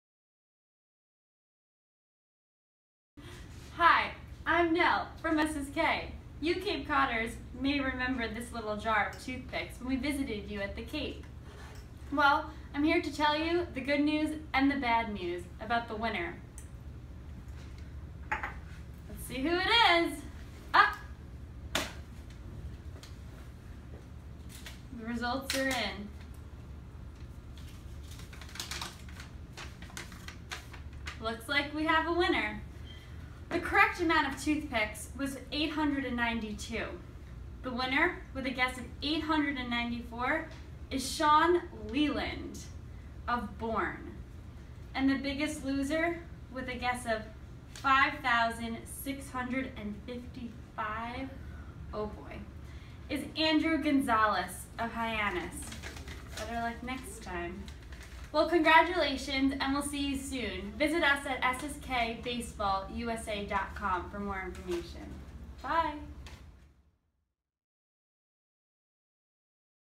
SSK Toothpick Count Challenge #1
Discover the winner of our 1st SSK Toothpick Count Challenge!